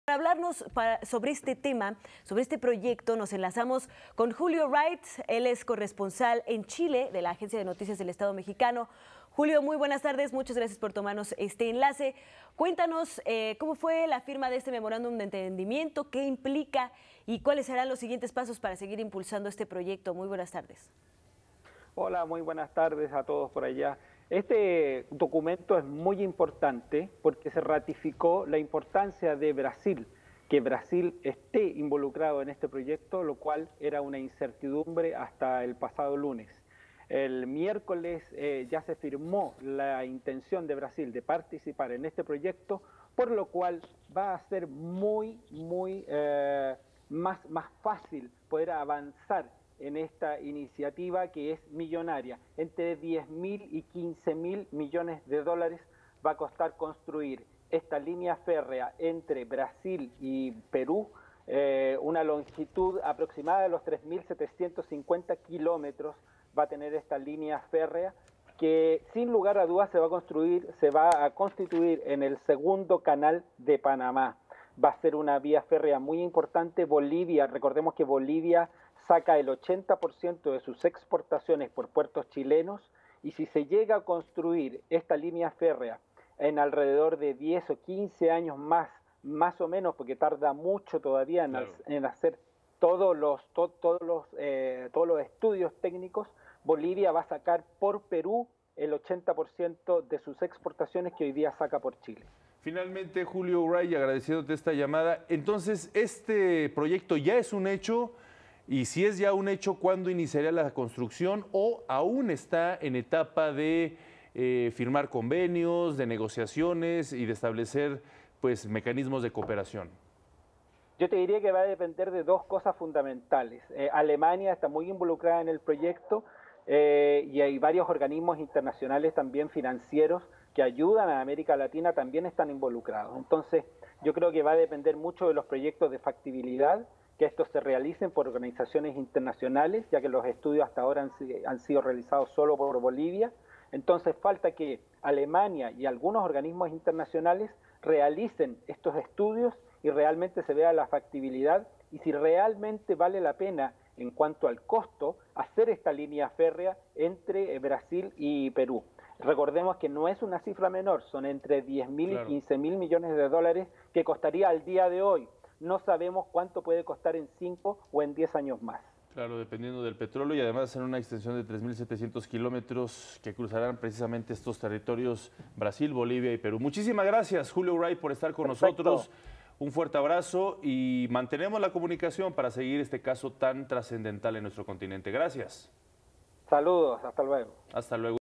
23/03/17 Julio Wright, corresponsal de Notimex en Chile
Los gobiernos de Bolivia y Alemania firmaron un memorándum de entendimiento para impulsar el Corredor Ferroviario Bioceánico, que unirá los Océanos Pacífico y Atlántico.
Para hablarnos sobre este proyecto nos enlazamos con Julio Wright, corresponsal de Notimex en Chile.